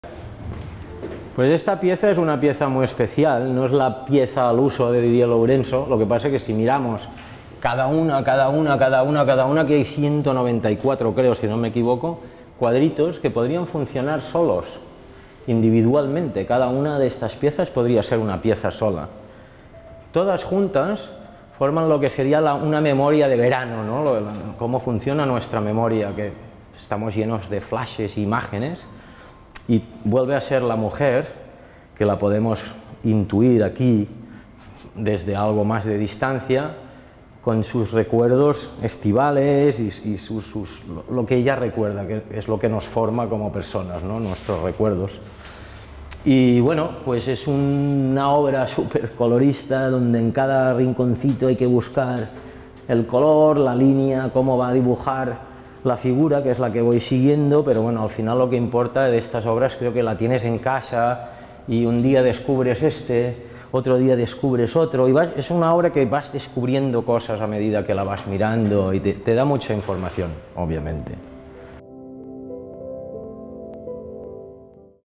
‘Soy yo’, de Didier Lourenço
Obra expuesta en ‘La primavera robada’, en la Galería Jordi Barnadas (del 17 de junio al 24 de julio de 2020)